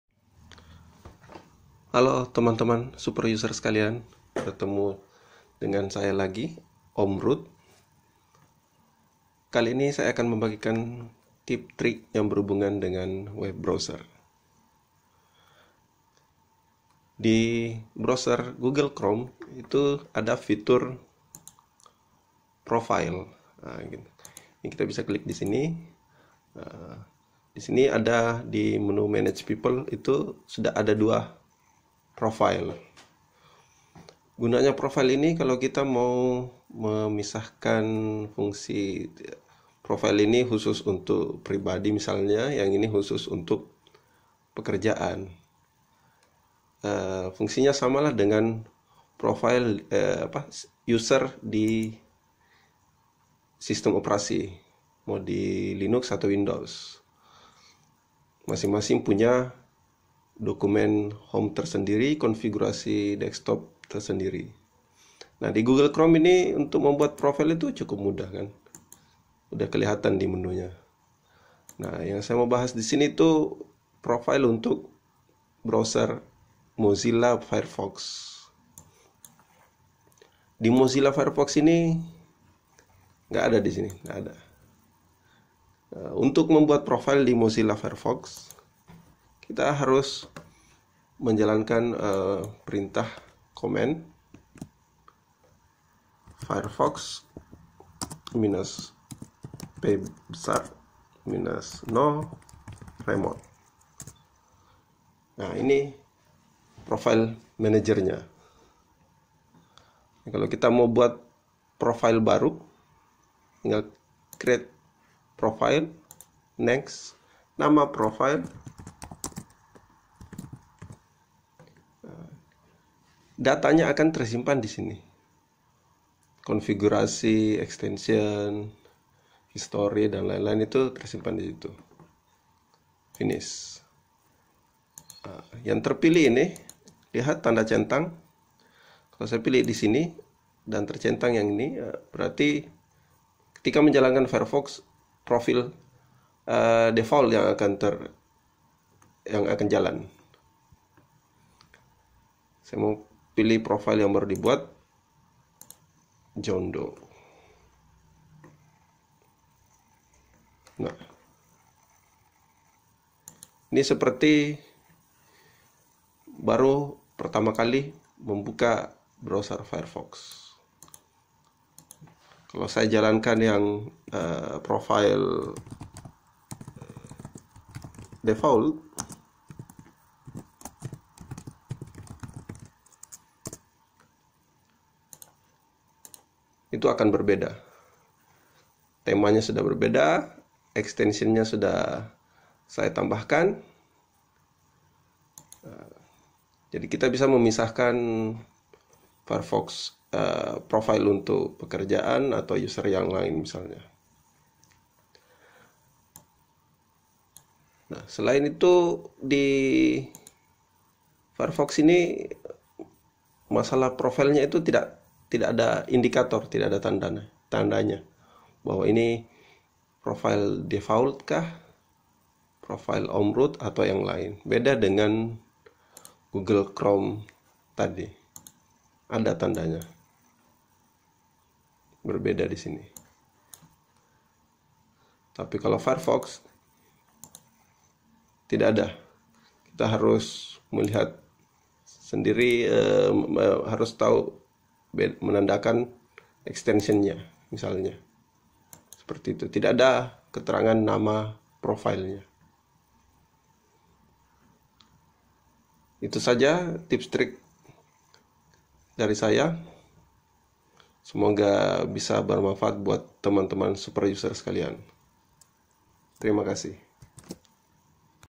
Cara Setting dan Menjalankan Multiple Profiles di Mozilla Firefox
Tutorial bagaimana cara setting dan menjalankan multiple profiles di Mozilla Firefox